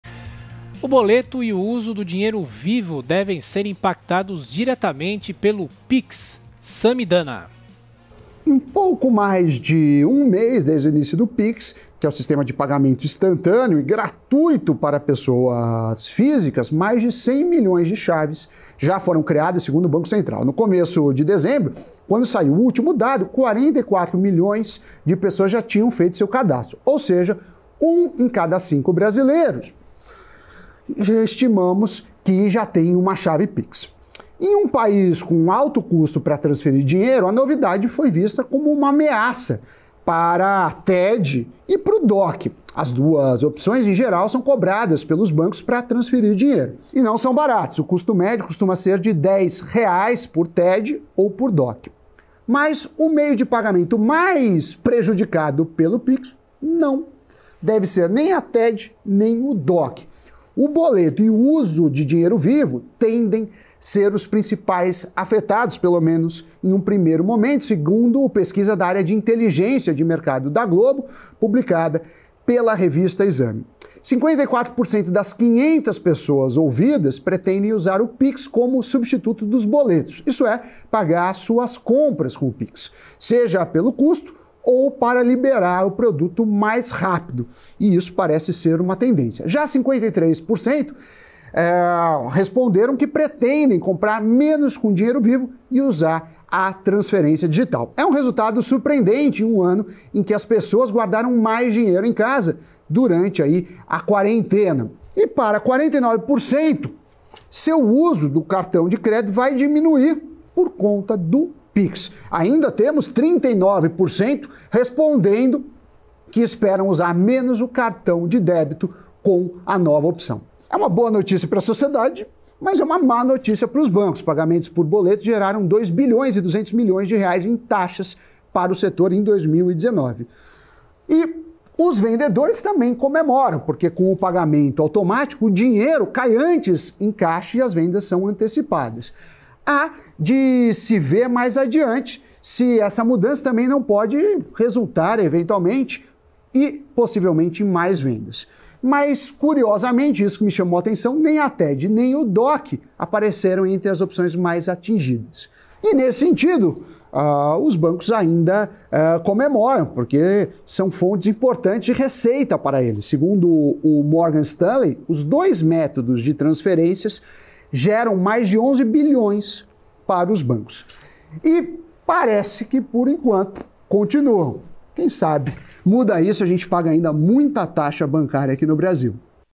Samy Dana: boletos e dinheiro vivo serão os mais afetados pelo Pix
Com menos de um mês em atividade, o Pix já se tornou a preferência da maioria dos brasileiros em relação aos modelos TED e DOC dos bancos. No entanto, Samy Dana revela que o pagamento via boletos e dinheiro vivo deverão ser os mais impactados.